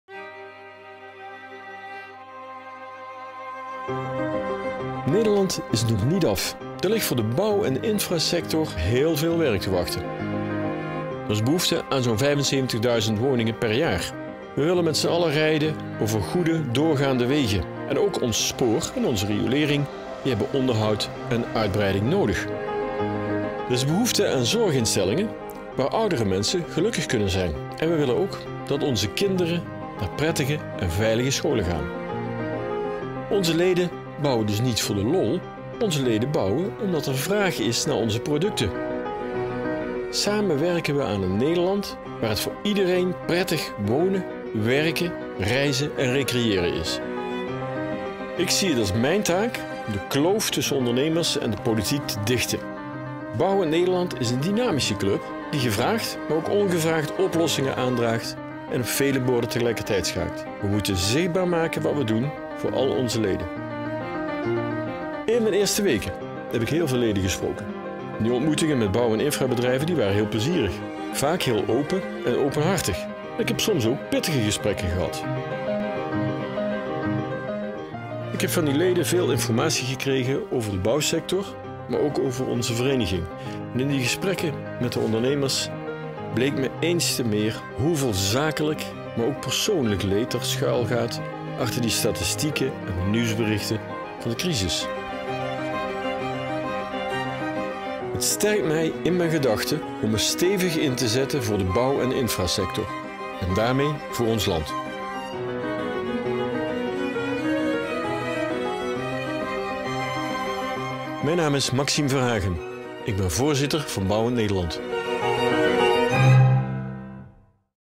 Maxime Verhagen zet zich in voor bouw en infra
Maxime Verhagen is de voorzitter van Bouwend Nederland. In deze film vertelt hij over het belang van de bouw- en infrasector voor Nederland. De duizenden kleine, middelgrote en grote bouw- en infrabedrijven bouwen en onderhouden Nederland. Zij zorgen ervoor dat we goed kunnen wonen, werken, recreëren en reizen. Bouwend Nederland is hun brancheorganisatie en wil hen zo zorgeloos mogelijk dit belangrijke werk laten doen. Maxime Verhagen zet zich elke dag in om de kloof tussen bouwondernemers en politiek te dichten. Zodat de leden van Bouwend Nederland hun belangrijk werk kunnen blijven doen.